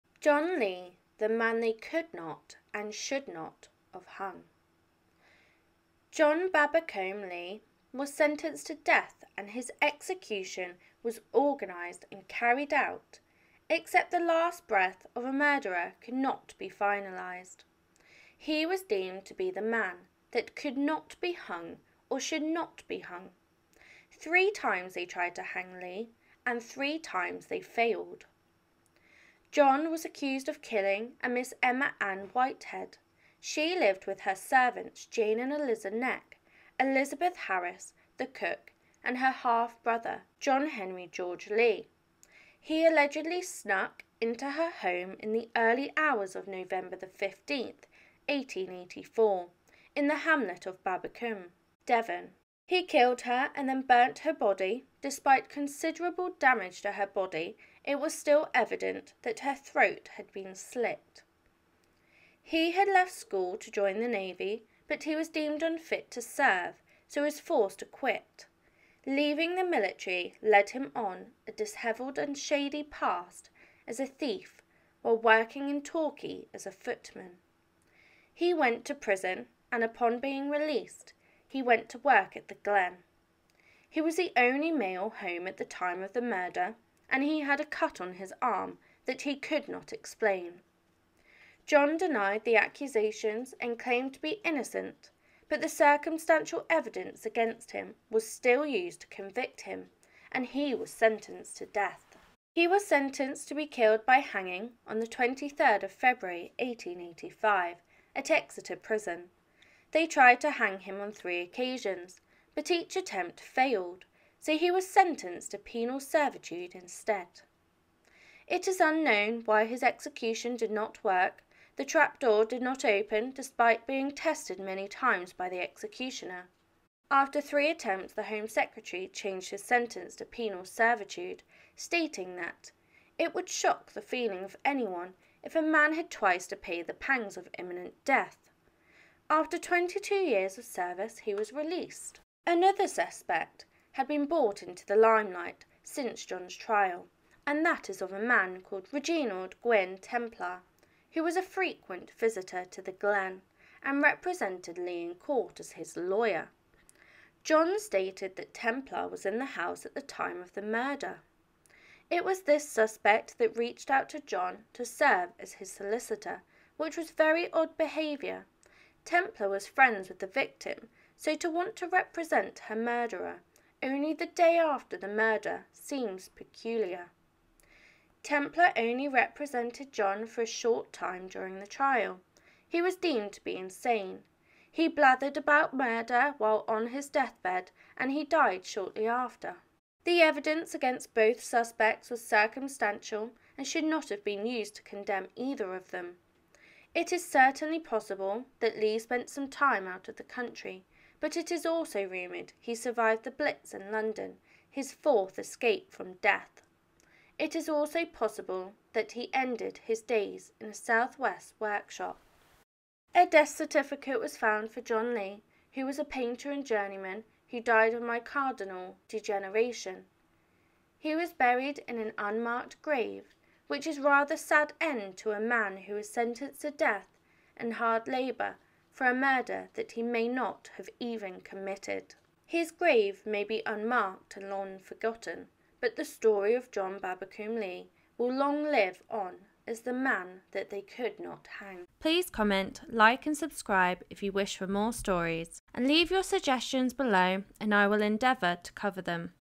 John Lee | The Man They COULDN'T Hang
John "Babbacombe" Lee was an Englishman famous for surviving three attempts to hang him for murder. Born in Devon, Lee served in the Royal Navy, and was a known thief.

In 1885, he was convicted of the murder of his employer, Emma Keyse, at her home at Babbacombe Bay near Torquay on 15 November 1884 with a knife.

The evidence was weak and circumstantial, amounting to little more than Lee having been the only male in the house at the time of the murder, his previous criminal record, and being found with an unexplained cut on his arm.

Despite this and his claim of innocence, he was sentenced to hang. Having survived three attempts at hanging, his sentence was commuted to life imprisonment. He became popularly known as "the man they couldn't hang".